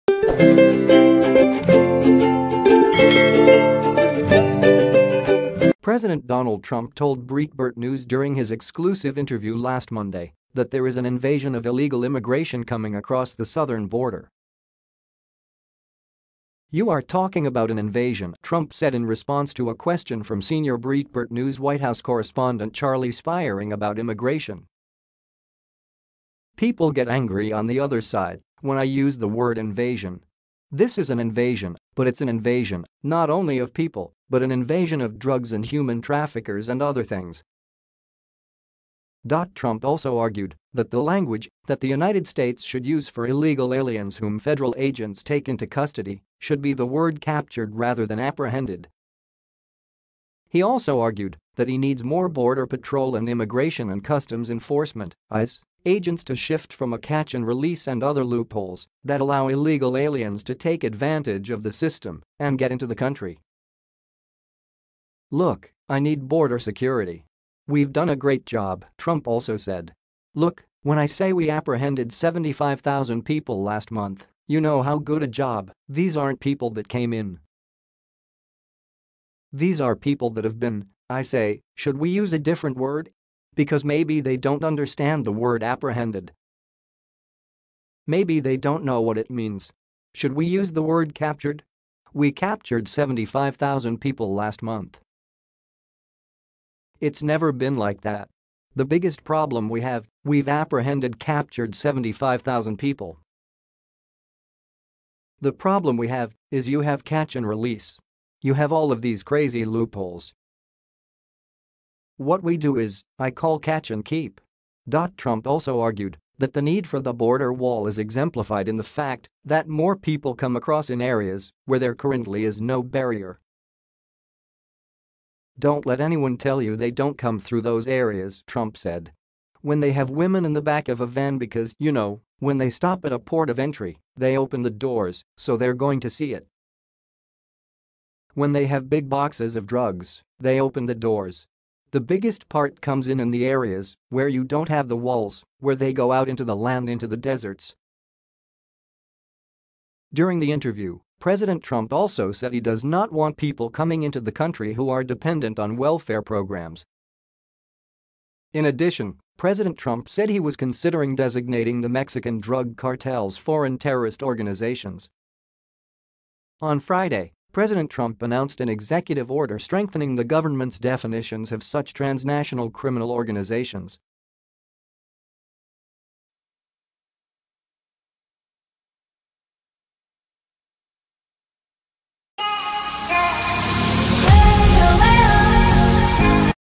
Donald Trump: US Has ‘Captured’ Thousands of Illegal Aliens
President Donald Trump told Breitbart News during his exclusive interview last Monday that there is an “invasion” of illegal immigration coming across the southern border. “You are talking about an in